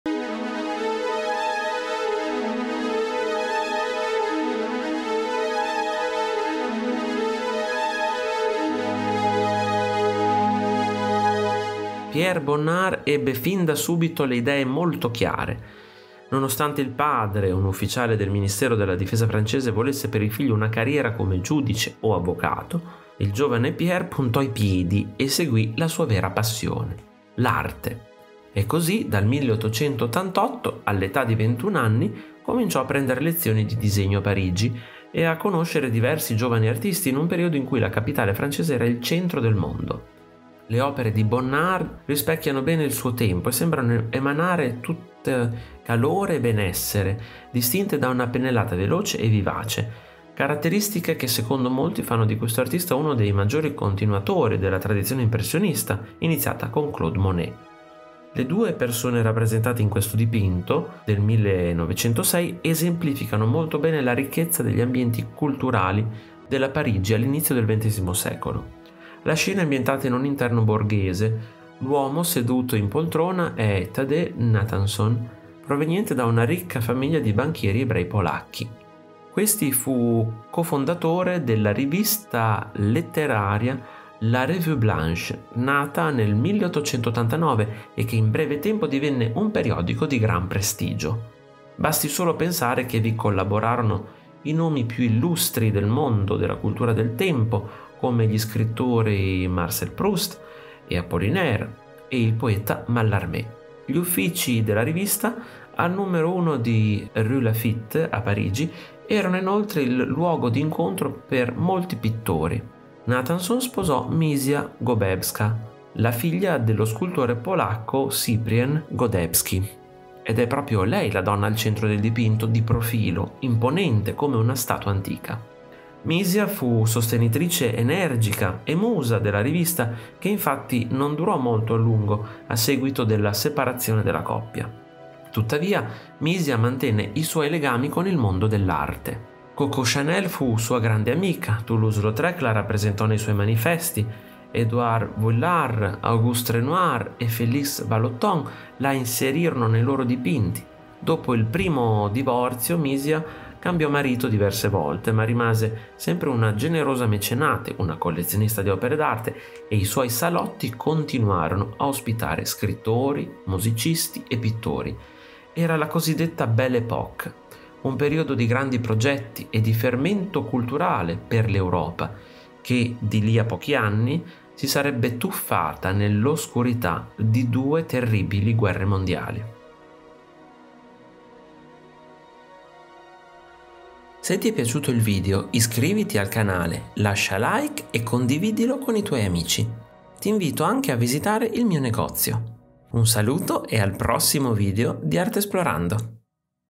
Pierre Bonnard | Thadee Natanson e Misia
L'audioquadro è un modo per conoscere i più grandi capolavori della storia dell'arte. In maniera semplice e in pochi minuti. Qui vi parlerò del ritratto di Thadee Natanson e Misia di Pierre Bonnard▼▼CONTINUA SOTTO▼▼

Pierre Bonnard, Thadee Natanson e Misia, 1906 (circa), museo Reina Sofia, Madrid

Pierre Bonnard ebbe fin da subito le idee molto chiare. Nonostante il padre, un ufficiale del ministero della difesa francese, volesse per il figlio una carriera come giudice o avvocato, il giovane Pierre puntò i piedi e seguì la sua vera passione: l’arte. E così dal 1888, all’età di ventun anni, cominciò a prendere lezioni di disegno a Parigi e a conoscere diversi giovani artisti in un periodo in cui la capitale francese era il centro del mondo.

LETTURE CONSIGLIATE